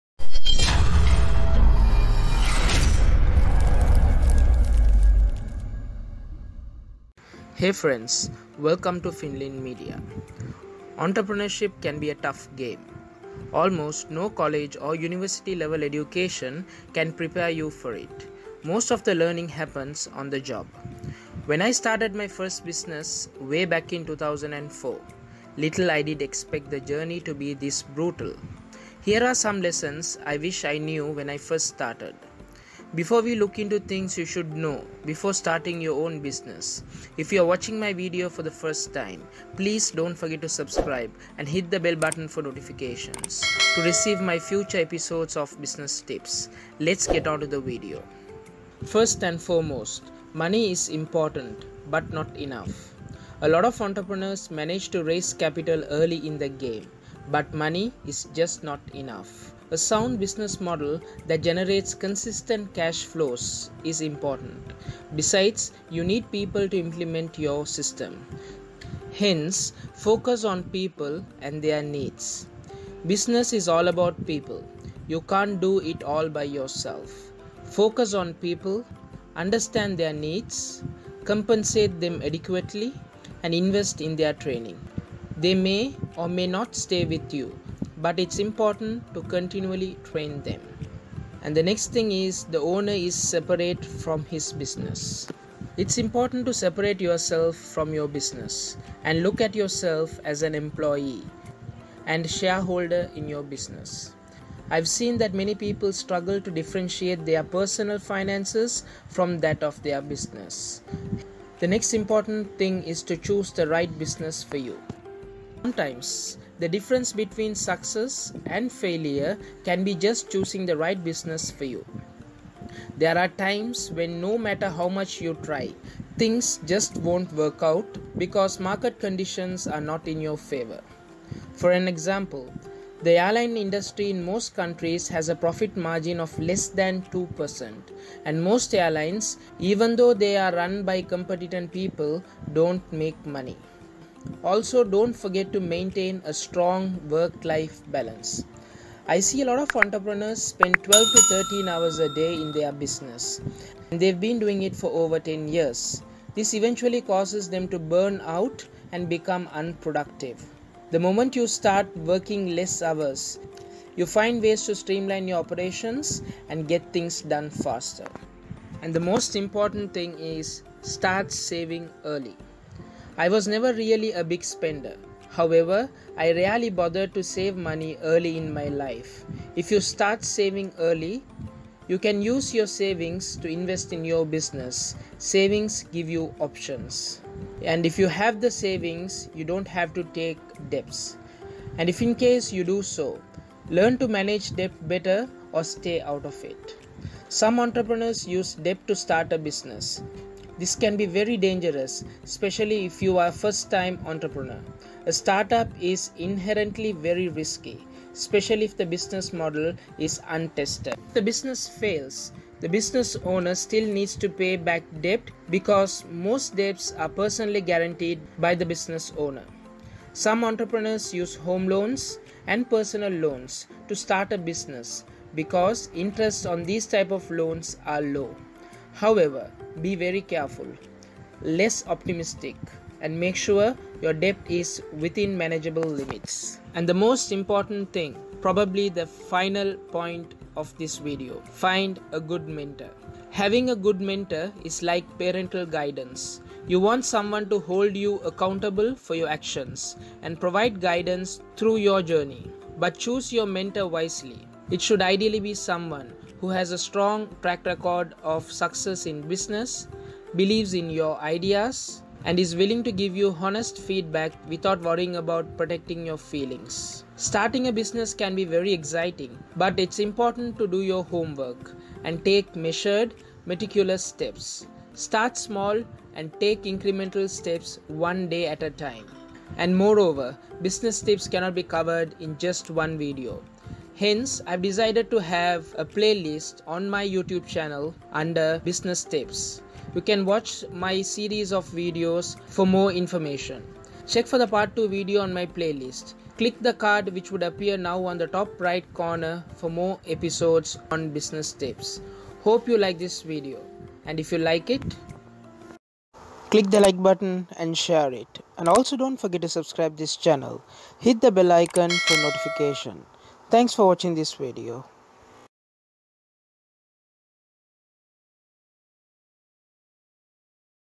DONT START A BUSINESS BEFORE YOU WATCH THIS PART 1 | English | Finlyn Media | NF
In this video you will learn about important things you should know before starting your own business. This is the Part 1 video.